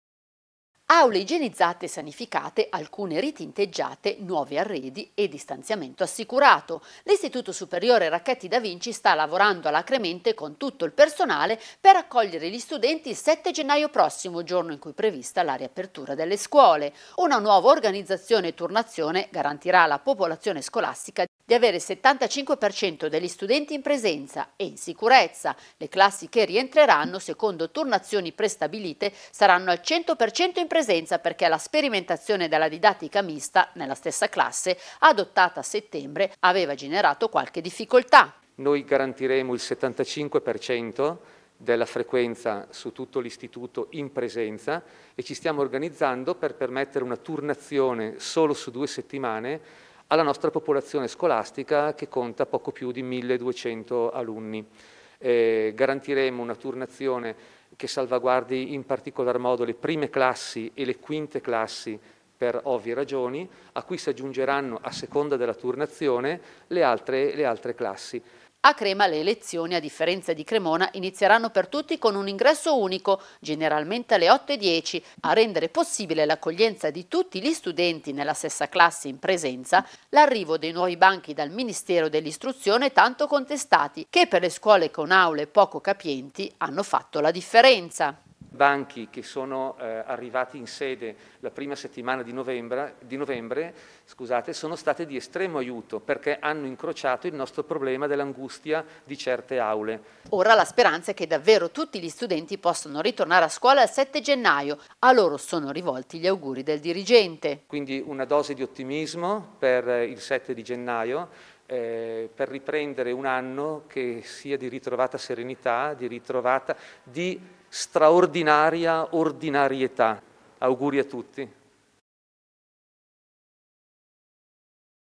Crema: il Racchetti-DaVinci pronto per il 7 gennaio
CREMA, L ‘ISTITUTO SUPERIORE RACCHETTI DA VINCI  È PRONTO AD ACCOGLIERE GLI STUDENTI IL 7 GENNAIO
 Si sta lavorando alacremente  all’ Istituto Superiore “Racchetti i Da Vinci” che comprende i licei classico, scientifico e linguistico  per accogliere gli studenti alla riapertura, prevista il 7 gennaio. Una nuova organizzazione e turnazione consentirà di avere il 75%  degli studenti  in presenza. Grazie all'arrivo dei nuovi banchi dal Ministero dell’ istruzione  sarà garantito il distanziamento